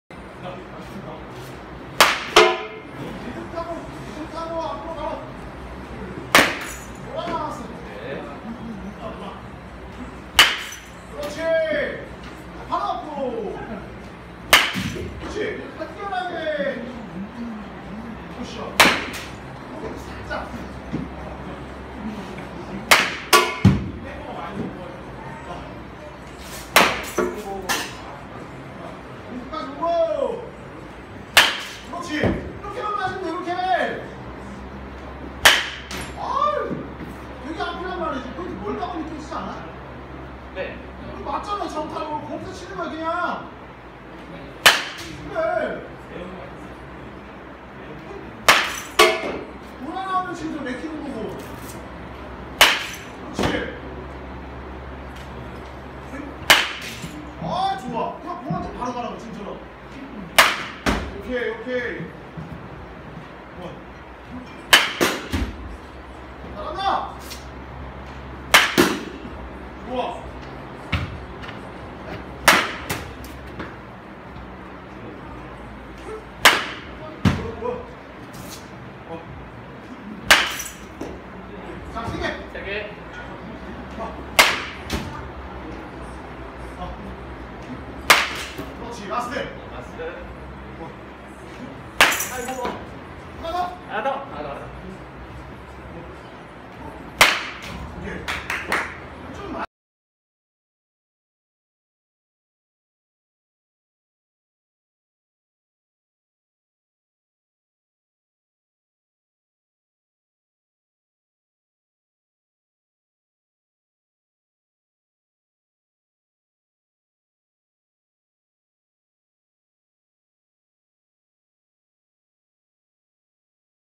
김현상님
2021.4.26 엠베이스볼 신설동점 김현상님 타격영상